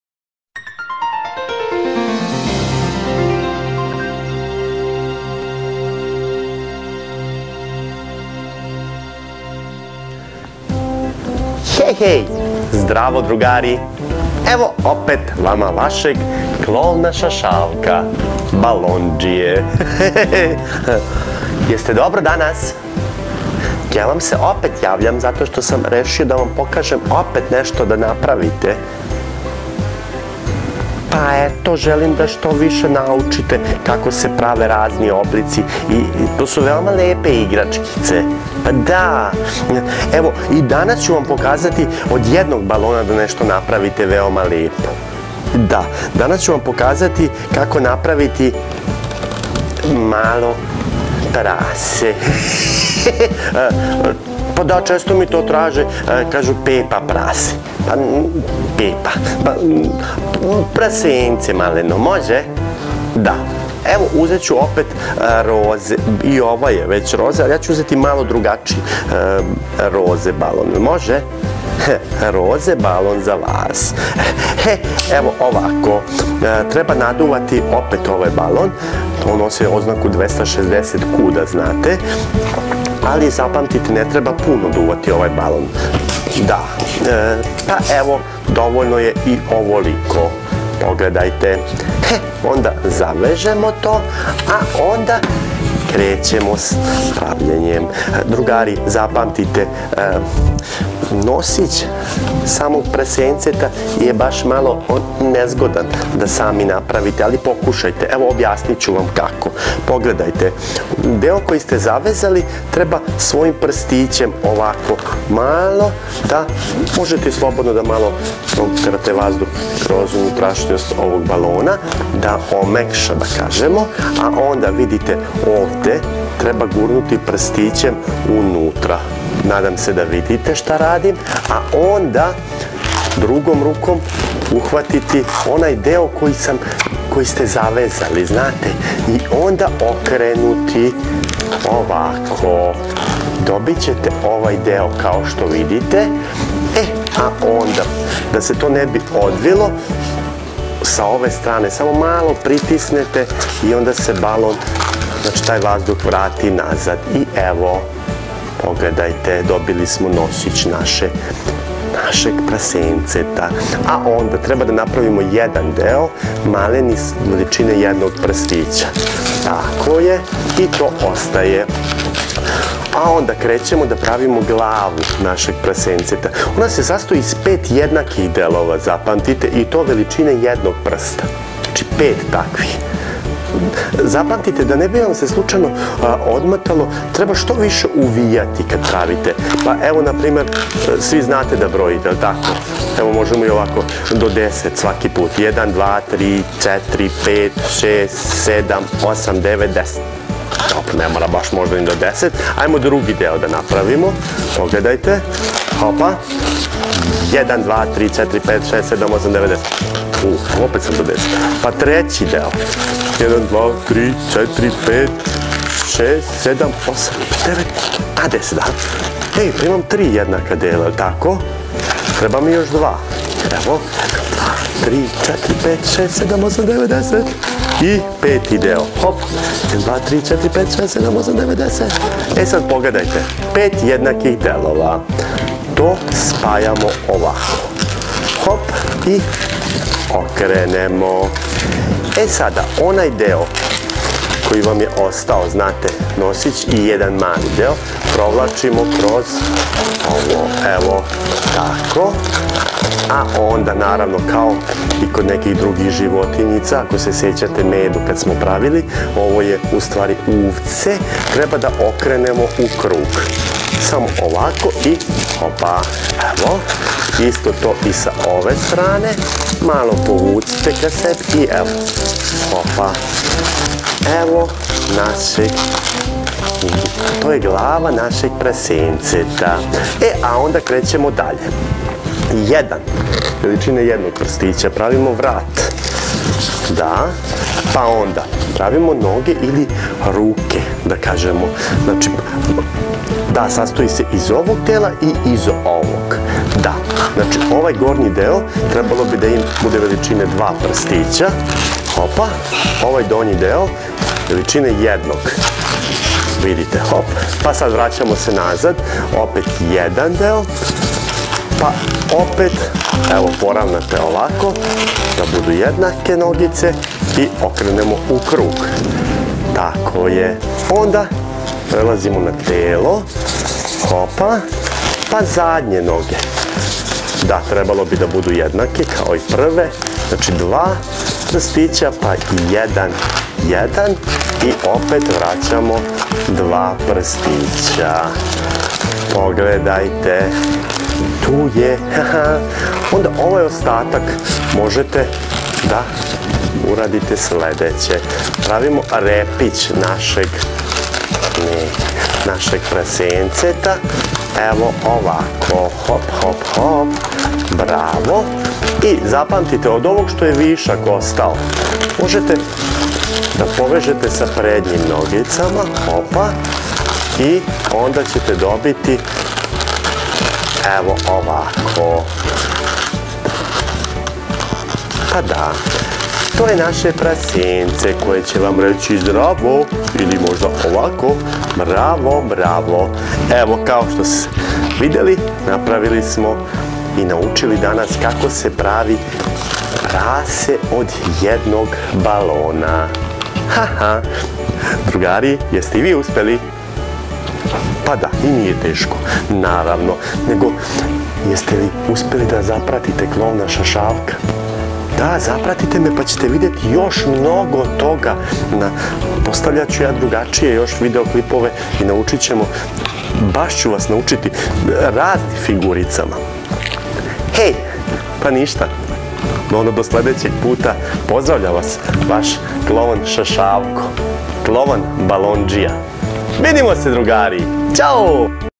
PRASE od jednog balona - Modeliranje balona #10
Naučite da pravite životinjice i razne figurice od balona.

U ovoj lekciji ćete naučiti kako da od jednog balona napravite PIG.

Learn how to make animals and figures from balloons.
This episode will show you how to make a basic balloon PIG.